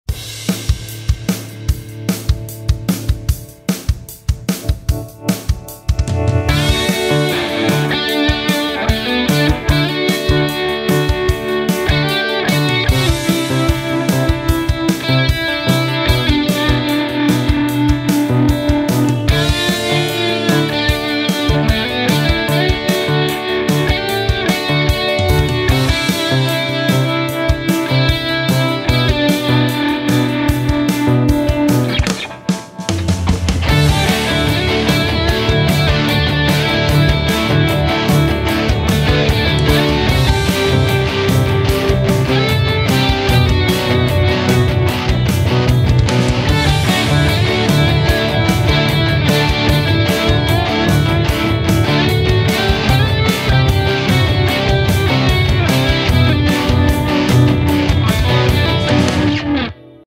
Some American Rock Tones in D Major
Writing a seemingly classic Rock vibe in D Major

You also have to be following the following IG accounts